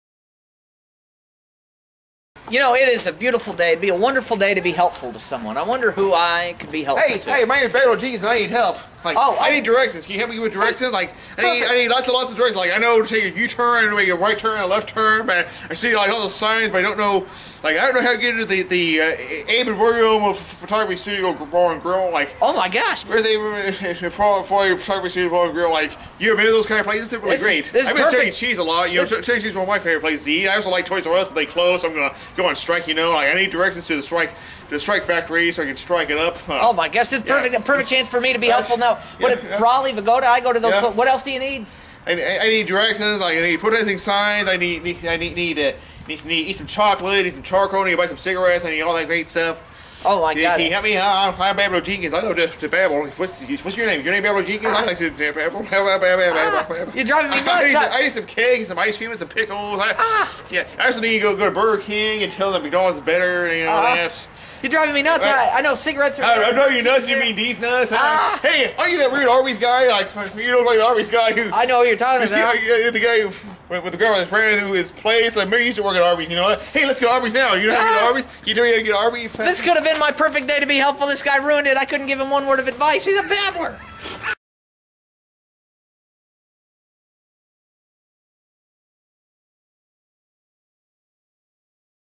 Babbler Jenkins Needs Directions
Babbler Jenkins asks a man to help him with directions but won't give the man a word in edgewise as he rambles on and goes off-track over what he really needs to know.